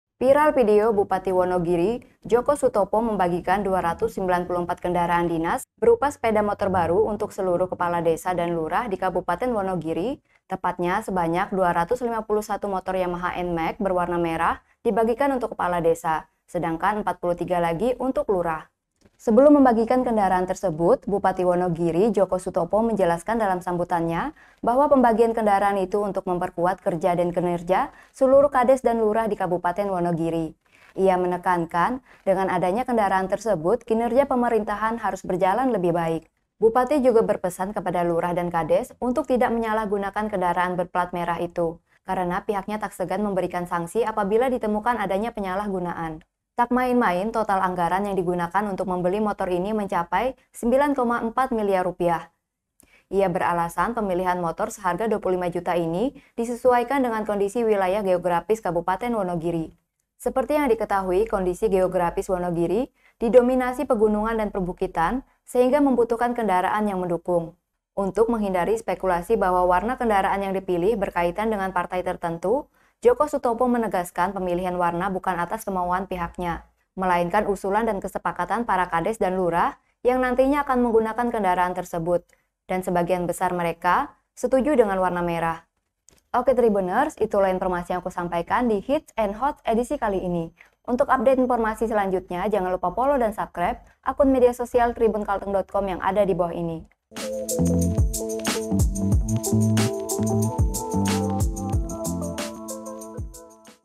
Bupati Wonogiri Bagi-bagi Ratusan Motor NMAX untuk Kades
Viral video Bupati Wonogiri Joko Sutopo membagikan 294 kendaraan dinas berupa sepeda motor baru untuk seluruh Kepala Desa dan Lurah di Kabupaten Wonogiri, di Pendoro Rumah Dinas Jabatan Bupati.

Tepatnya, Sebanyak 251 motor Yamaha Nmax berwarna merah dibagikan untuk Kepala Desa, sedangkan 43 lagi untuk Lurah.